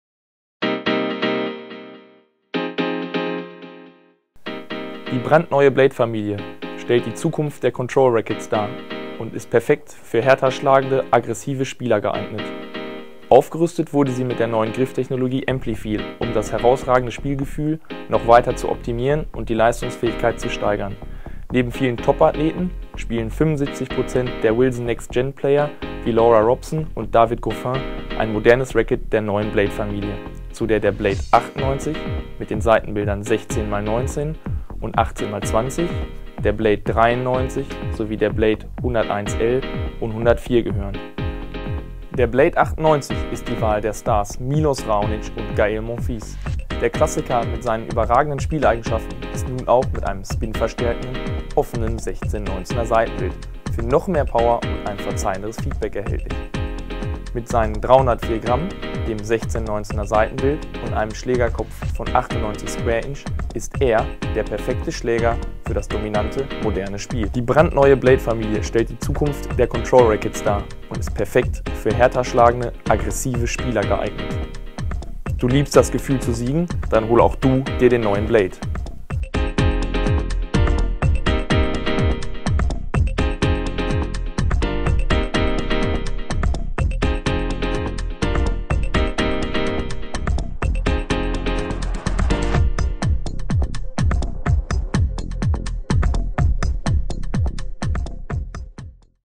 Tennis-Point.de - Wilson Blade 98 16x19
Die neue Generation der Kontrolle!  Das Racket auf das auch Milos Raonic vertraut!  Der Profi-Rahmen für den aggressiven Spielertyp!  Eigenschaften: Das mittlere Gewicht, 304 g unbespannt, und die gute Ausgewogenheit, Balancepunkt 32,5 cm, sichern eine gute Manövrierbarkeit des Rackets, liefern aber gleichzeitig die Voraussetzungen für eine gute Schläger- bzw. Ballbeschleunigung. Die im Vergleich zum Blade 93 größere Schlägerkopffläche, 632 cm2, bietet einen größeren Sweetspot. Aufgrund der genannten Eigenschaften zeichnet sich das Racket durch eine sehr gute Kombination aus Power und Kontrolle aus. Das spinverstärkende, offene Saitenbild (16/19) optimiert die Spineigenschaften des Schlägers und liefert ein verzeihenderes Feedback.  Technologien: Durch die BLX-Technologie und die Verwendung von Basalt-Fasern in einer Matrix sowie der Verarbeitung von Basalt im Griff (Amplifeel-Technologie), werden extreme Schwingungen verringert und das Spielgefühl verbessert. Eine an den Spielertyp angepasste Schäumung des Griffes und weiche Griffkappen ermöglichen ebenfalls ein sehr gutes Feeling. Durch die Double Hole-Technologie (vergrößerte Ösen), werden mehr Saitenbewegungen ermöglicht und so der Sweetspot vergrößert.